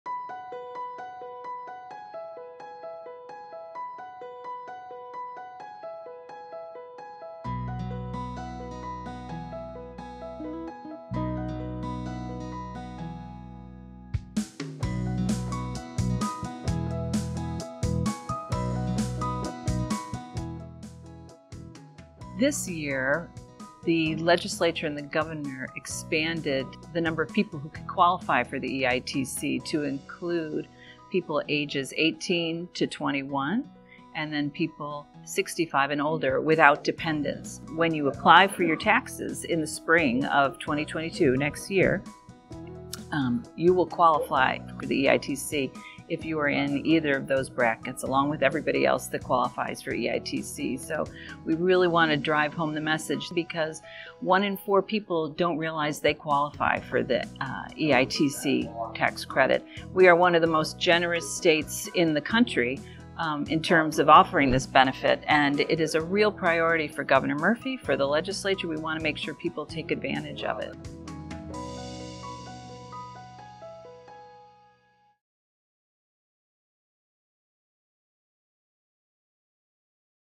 Back in Your Pocket: Earned Income Tax Credit
The New Jersey Earned Income Tax Credit is putting money back into the pockets of hard working New Jerseyans. Treasurer Elizabeth Maher Muoio explains how the tax credit has been expanded. To see if you're eligible to receive the New Jersey Earned Income Tax Credit,